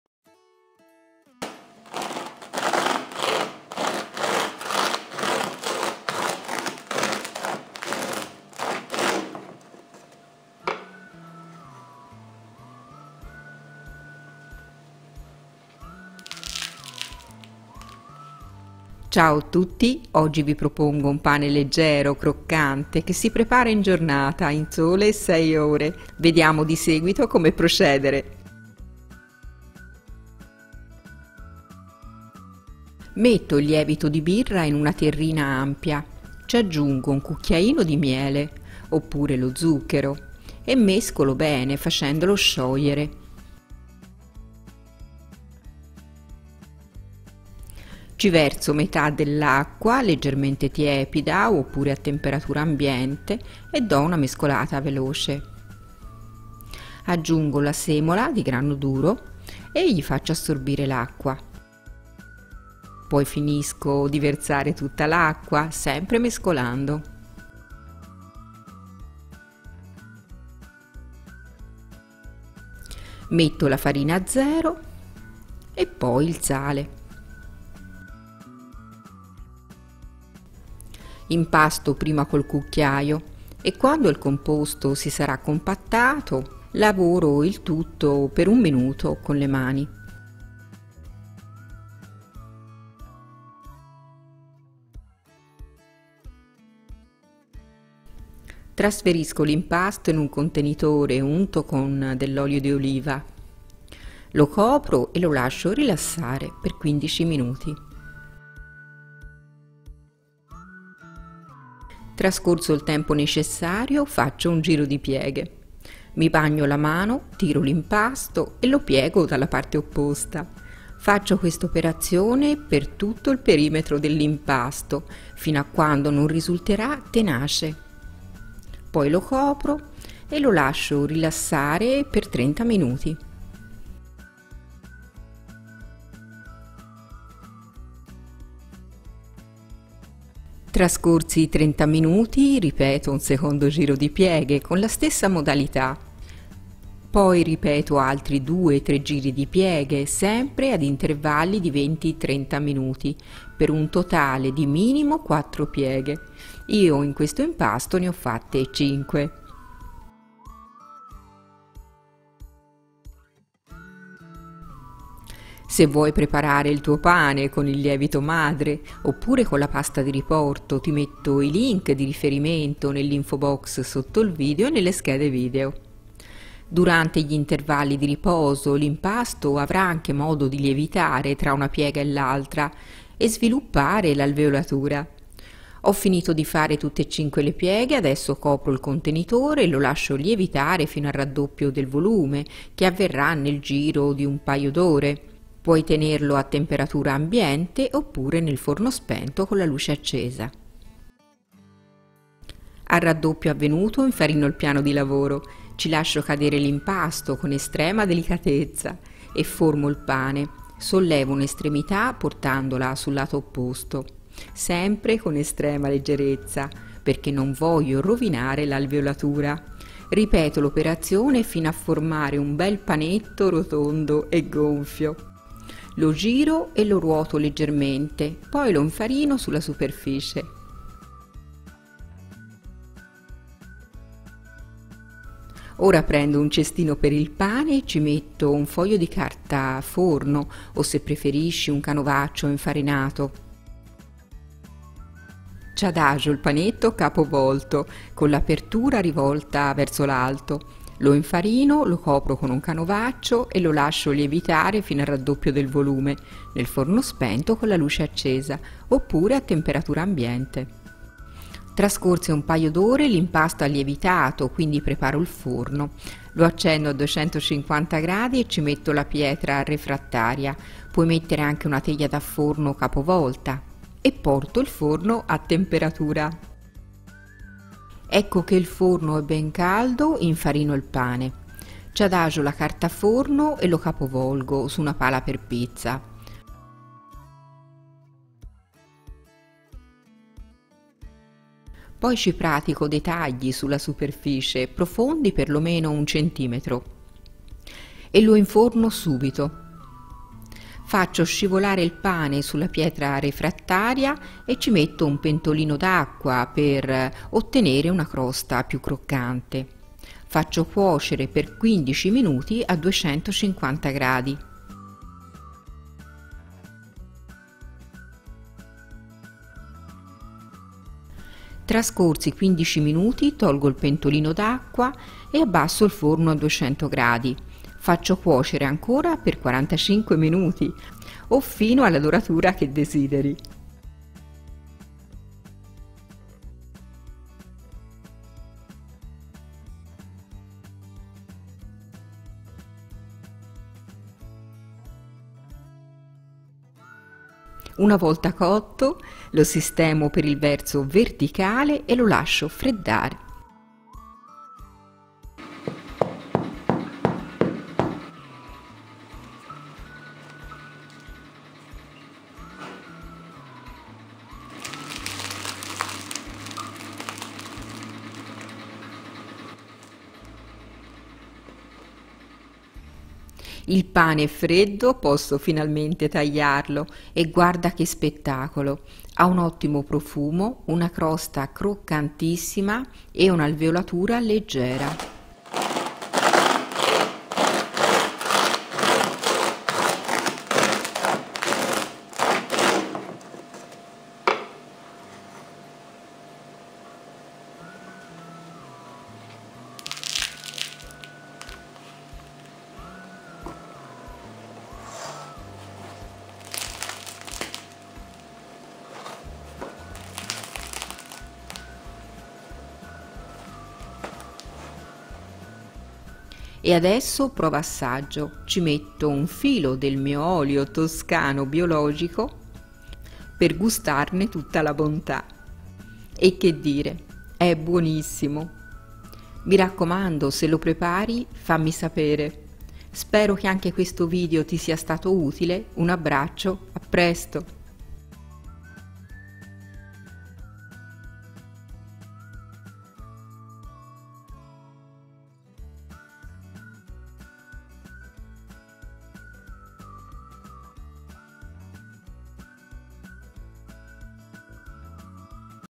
Pane leggero e croccante in 6 ore I Le Ricette di TerroreSplendore
Salve a tutti, oggi ho preparato un pane leggero e croccante in 6 ore, con il lievito di birra, per chi non ha a disposizione il lievito madre. Il risultato è stato eccellente, un pane profumato e ben alveolato, ottimo con affettati e formaggi, oppure con burro e marmellata.

Ingredienti:
-300 gr di farina 0 (12gr di proteine)
-200 gr di semola di grano duro
-4 gr di lievito di birra fresco
-1 cucchiaino di miele
-10 gr di sale
-350 gr di acqua